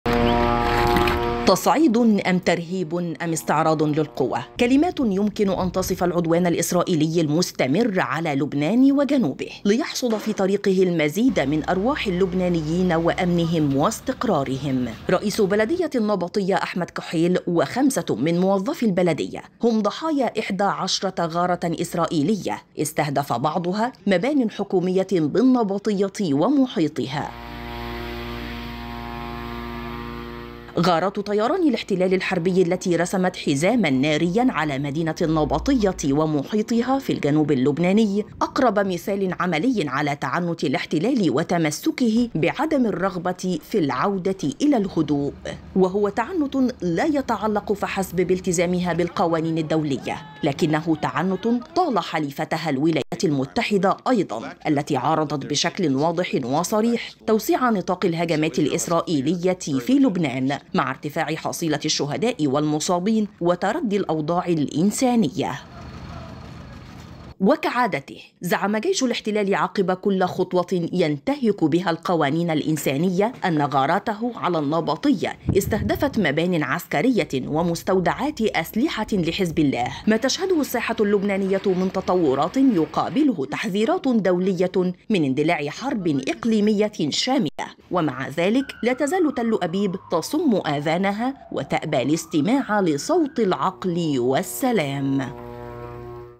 التصعيد الإسرائيلي مستمر.. تل أبيب تتجاهل دعوات العقل والسلام
17/10/2024